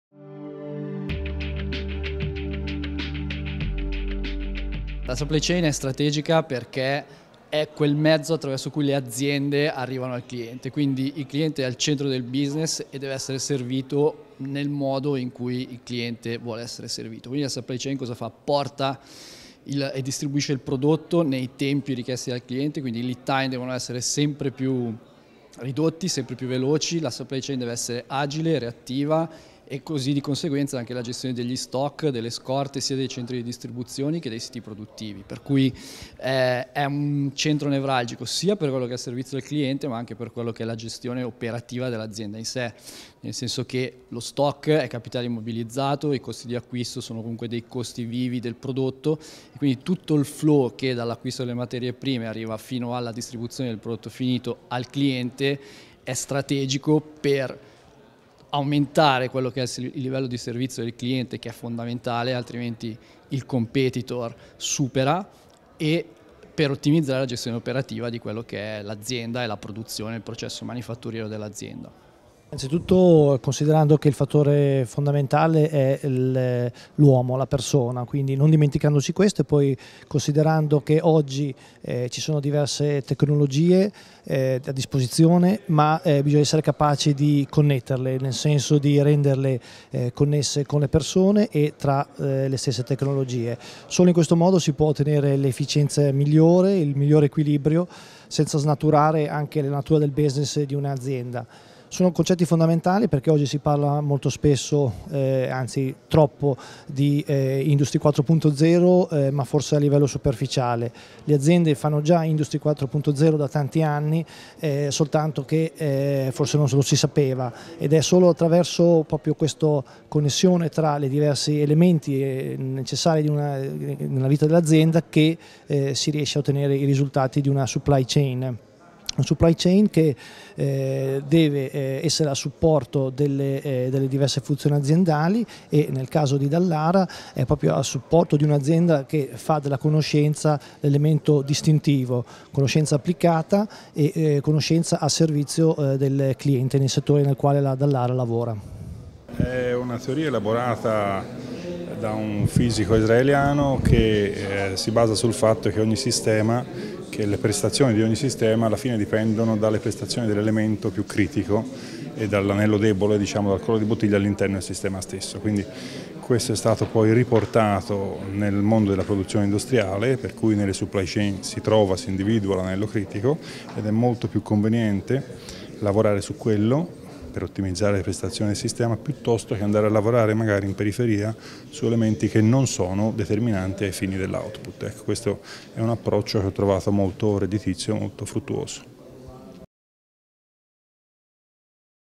Supply Chain, una leva strategica - Convegno Fabbrica Futuro Bologna 2016
Interviste a:

- Dino Alpi, supply chain manager - ATLAS COPCO BLM
- Alessandro Berzolla, chief operations officer - DALLARA